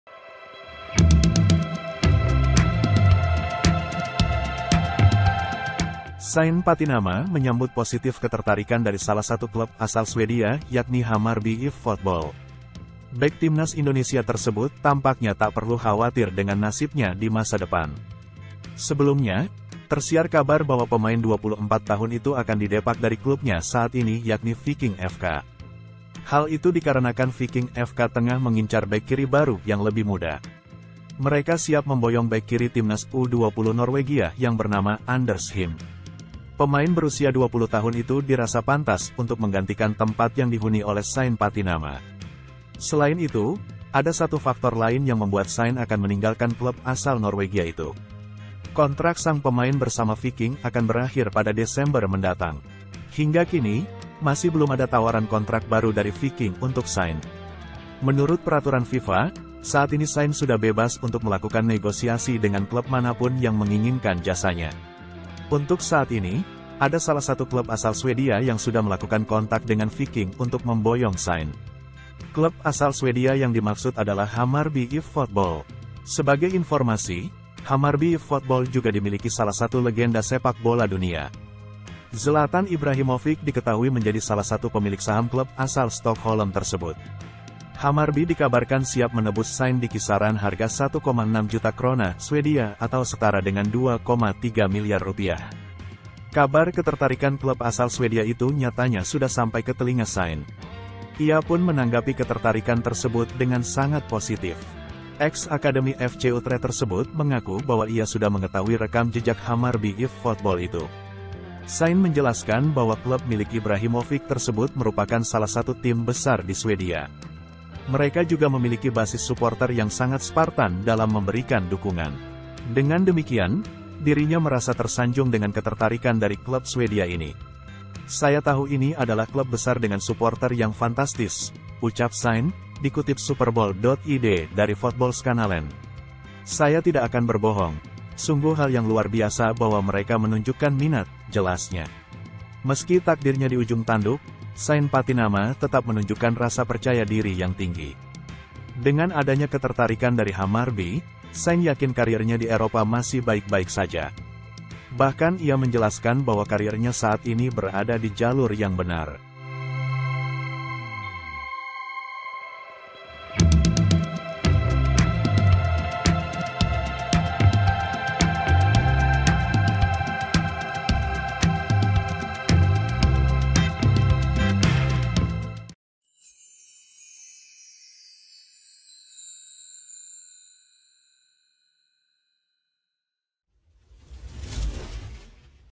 BERITA TIMNAS | Diminati Klub Swedia Milik Zlatan Ibrahimovic, Shayne Pattynama Langsung Caper
Shayne Pattynama menyambut positif ketertarikan dari salah satu klub asal Swedia yakni Hammarby IF Fotboll.
Bek Timnas Indonesia tersebut tampaknya tak perlu khawatir dengan nasibnya di masa depan.

(HDK)